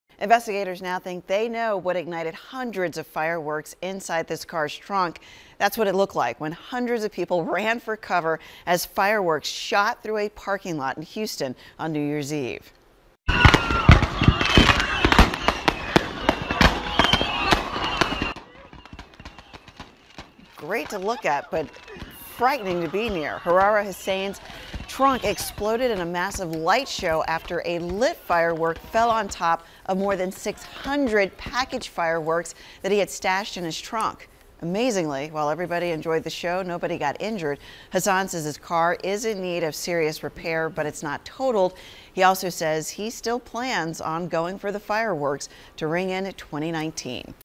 Fireworks explode in car trunk
Fireworks explode in car trunk. Tamala Edwards reports during Action News Mornings on January 3, 2018.